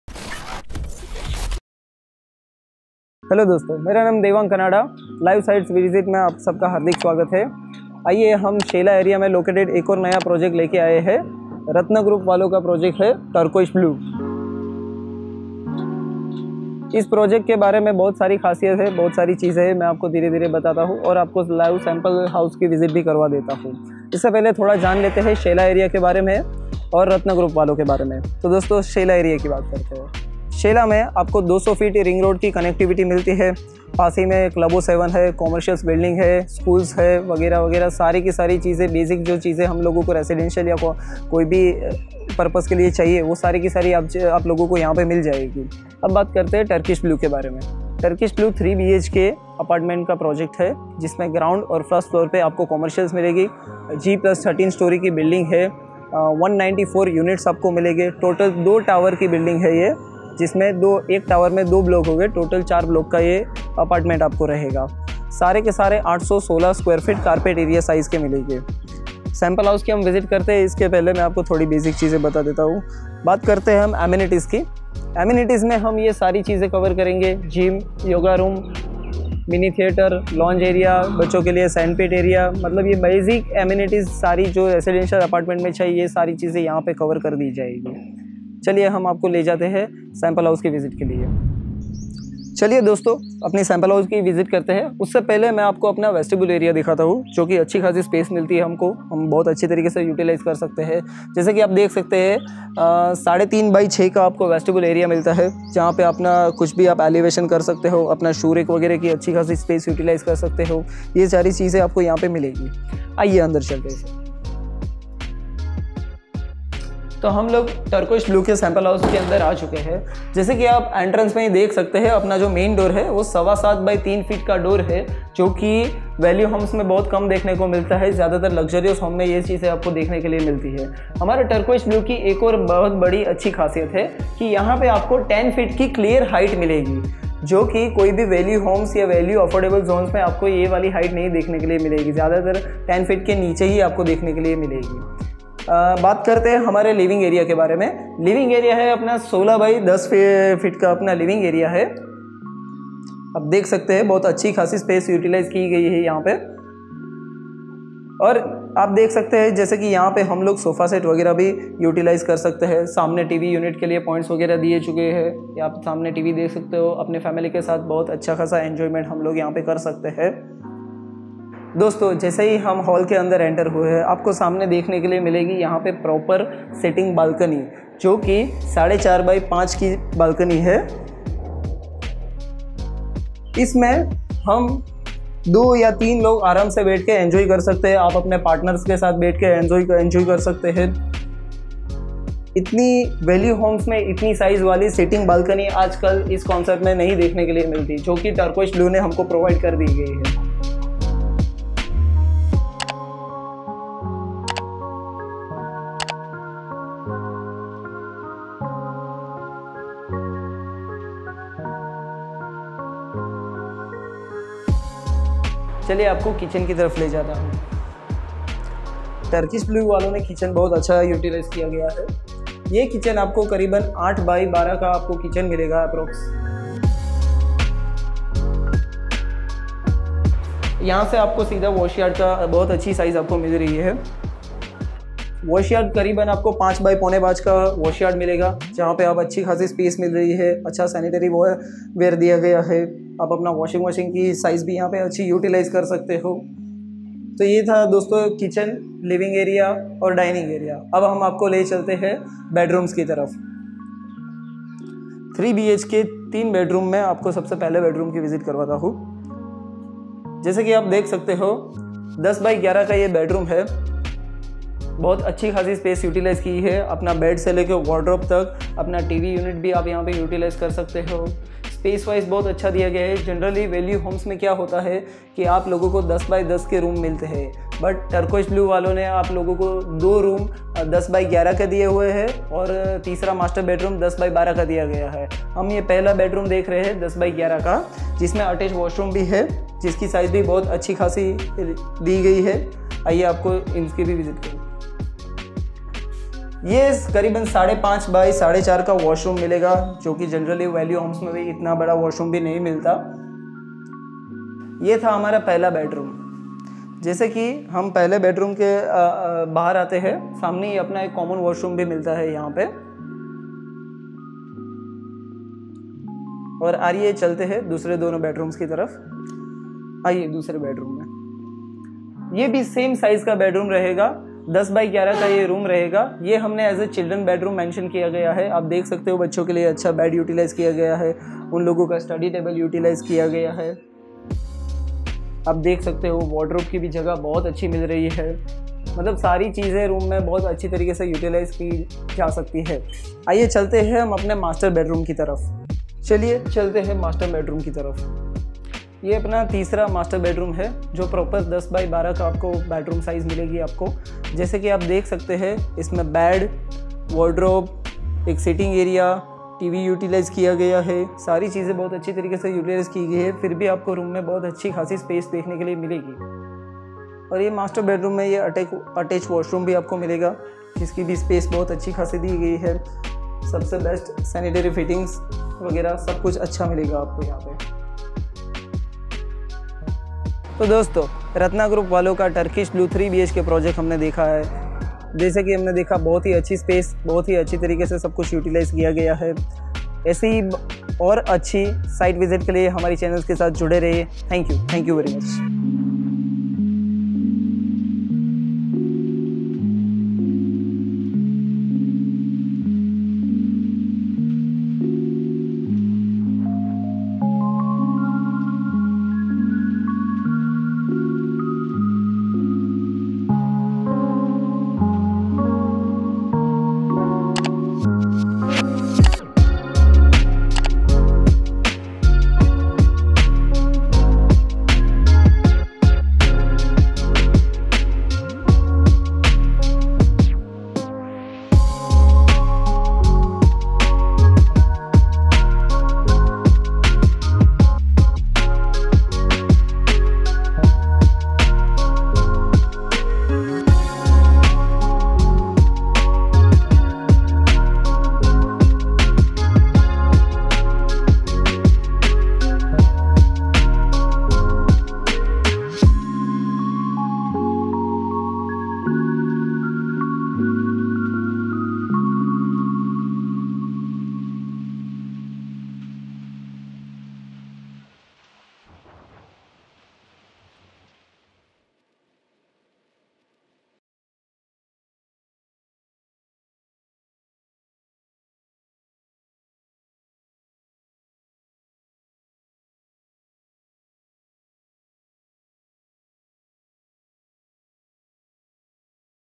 Turquoise Blu | 3 BHK Value Homes Apartment Project | at Shela, Ahmedabad | Ratna Group | Best Deals
Ratna Group have come up with a 3BHK  residential property, “Turquoise BLU” at Shela, SP Ring Road, Ahmedabad.

Here are some advantages of “Turquoise BLU”
We gave special attention to design from the inside out, taking the fragments of life within buildings and assembling them to design spaces that work effortlessly and fit together seamlessly. From these simple, functional spaces derive an honest and elegant aesthetic.

Location
Turquoise blu is located in the peaceful and fast-developing area of Shela & is extremely well connected to the South Bopal, SP Ring Road & SG Highway with large multiple roads. It has a unique locational advantage to west Ahmedabad’s fascinating areas, with proximity to restaurants, schools, malls, hospitals and more.

Landmark Nearest to Project

1) Nearest to Club o7 ( Gujarat biggest Club).
2) Upcoming 70000 sq.yard Auda Garden with the lake.
3) Well Planned AUDA auditorium.
4) Shanti Business School ( By Chiripal Group)
5) Krishna-Shalby Hospital

Planning Perfection:
1)  Well sizes 3 bedrooms & 3 Toilets.
2) 15 ft long platform in kitchen
3) Dedicated space for Washing machine & refrigerator
4) 10 ft clear height & Many more.